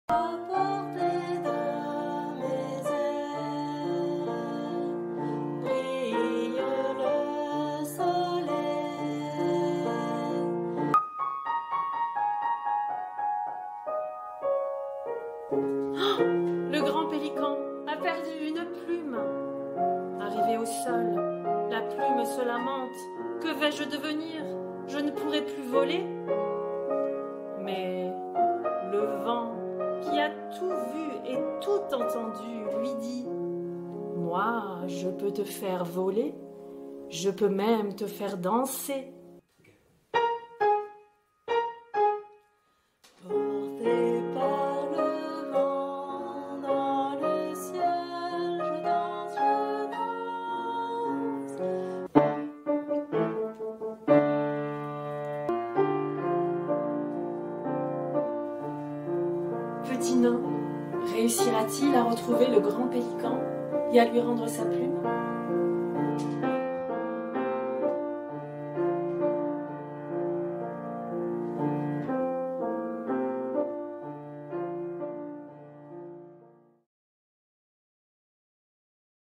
Petit Nain à la mer
L'été est là. Les enfants vont à la mer. Petit nain aussi !

Châteaux de sable, crabes, coquillages, plumes et voiliers sont au rendez-vous. Une journée à la plage remplie de jeux, de rencontres et d'aventures.